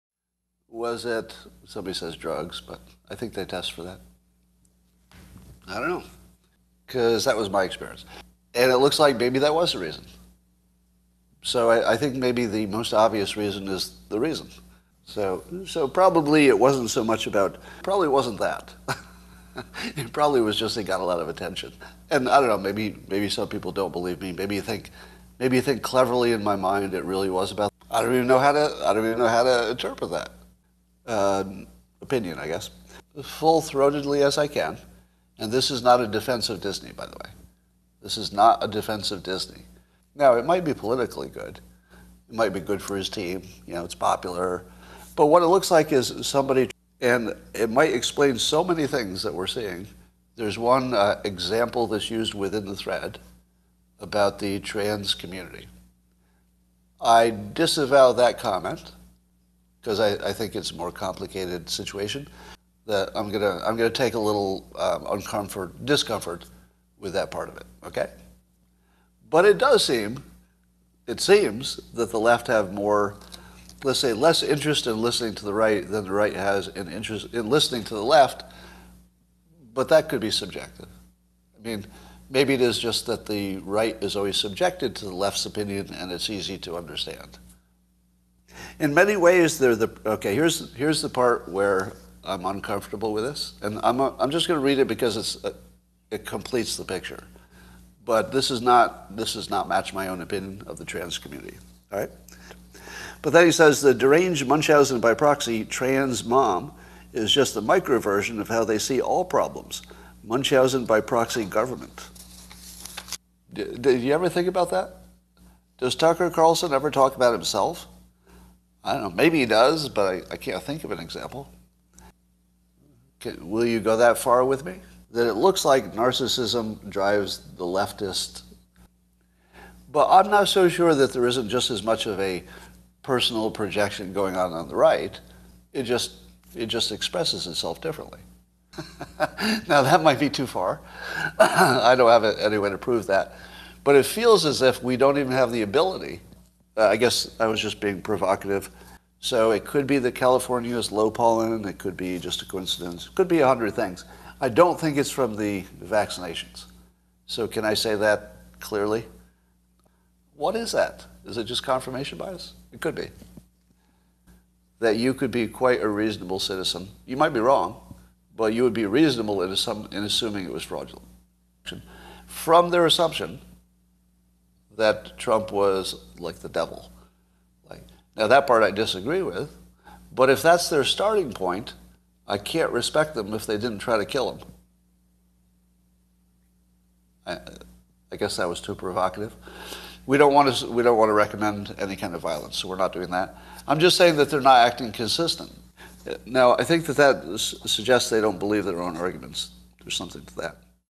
SCOTT ADAMS Mister Persuasive, Really
From 5/11/22 the poor man has a very hard time taking a position on any thing and he consider's himself a Master Persuader in Trump's class. I think possibly he's wrong but then it could be who knows, fact check me on that, it could be.